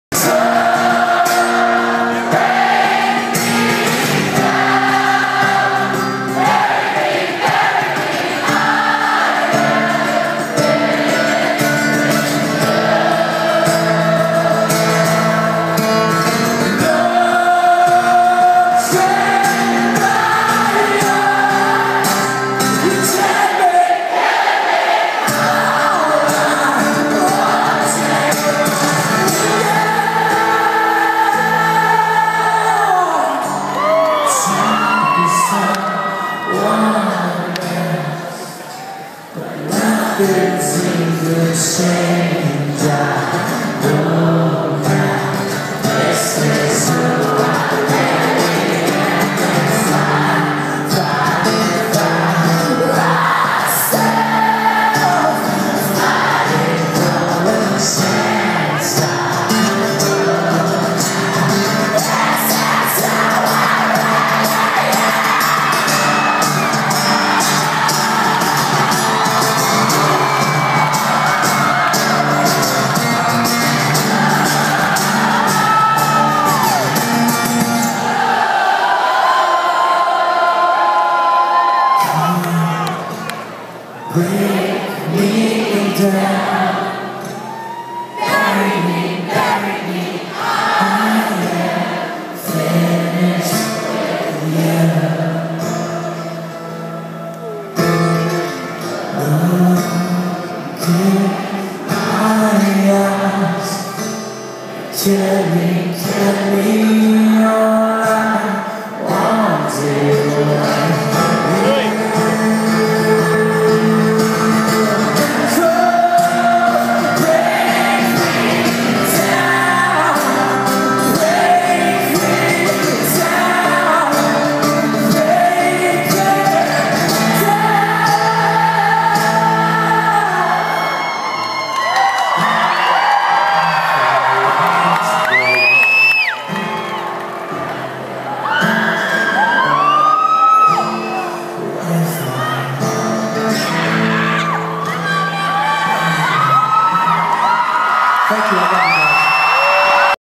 Jared Leto Hammerstein Ballroom
Video I took on iPhone of Jared Leto at the Hammerstein Ballroom